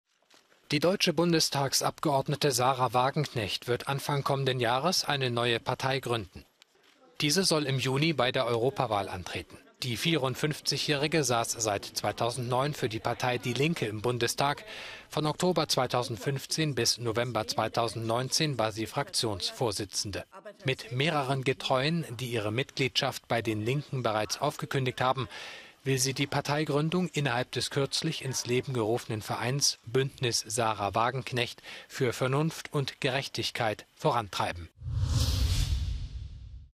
"Fischen am rechten Rand": Es hagelt Kritik gegen Wagenknecht-Partei
An diesem Montag hat die Noch-Linken-Politikerin Sahra Wagenknecht die Gründung ihrer neuen Partei angekündigt. Viele halten ihr Projekt für "konservativ".